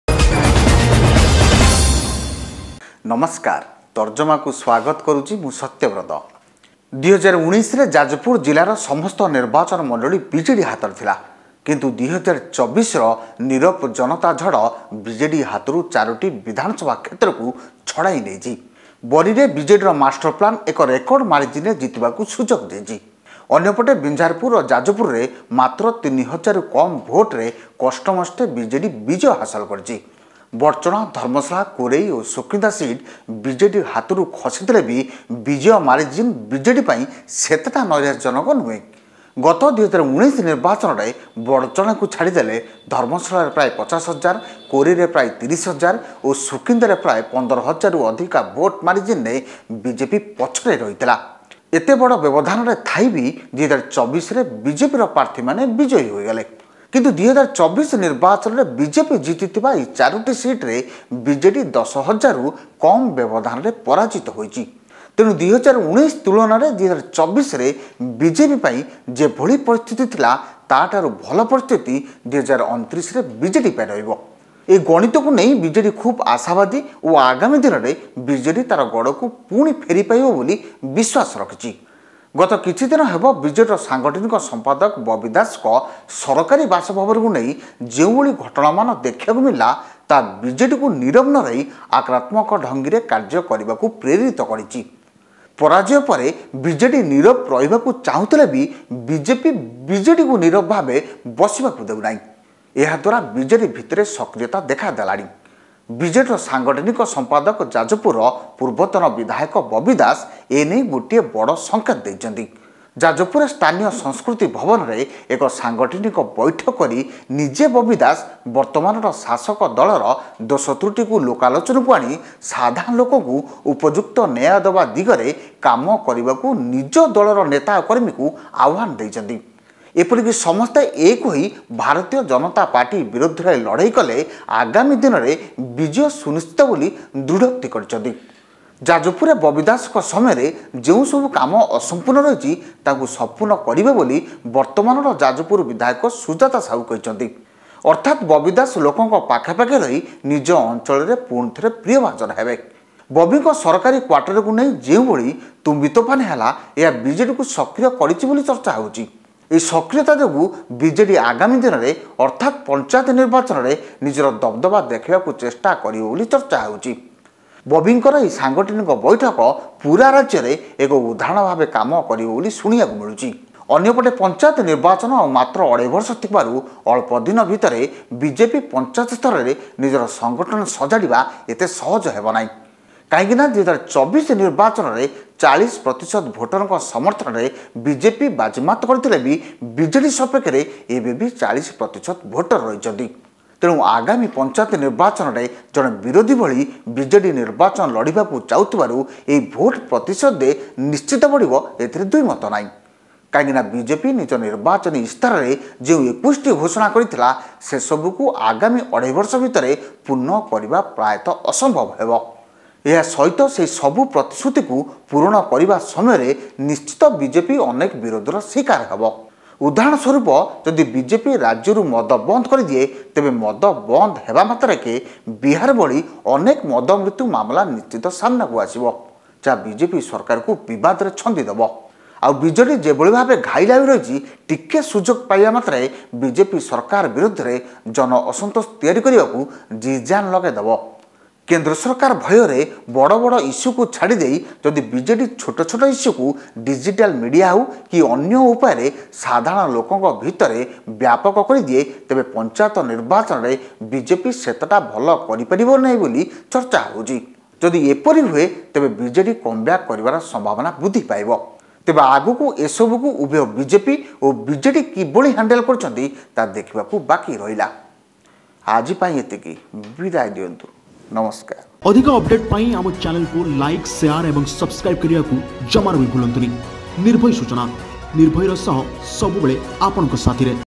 TARZAMA : ପୁଣି ଘର ସଜାଡ଼ିଲେ ବବି | Nirbhay Suchana
୨୦୧୯ରେ ଯାଜପୁର ଜିଲ୍ଲାର ସମସ୍ତ ନିର୍ବାଚନମଣ୍ଡଳୀ ବିଜେଡି ହାତରେ ଥିଲା । କିନ୍ତୁ ୨୦୨୪ର ନୀରବ ଜନତା ଝଡ ବିଜେଡ ହାତରୁ ୪ଟି ବିଧାନସଭା କ୍ଷେତ୍ରକୁ ଛଡାଇନେଇଛି । ବରୀରେ ବିଜେଡିର ମାଷ୍ଟର ପ୍ଲାନ ଏକ ରେକର୍ଡ ମାର୍ଜିନରେ ଜିତିବାକୁ ସୁଯୋଗ ଦେଇଛି । ଅନ୍ୟପଟେ ବିଞ୍ଝାରପୁର ଓ ଯାଜପୁରରେ ମାତ୍ର ୩ହଜାରରୁ କମ୍ ଭୋଟରେ କଷ୍ଟେ ମଷ୍ଟେ ବିଜେଡି ବିଜୟ ହାସଲ କରିଛି ।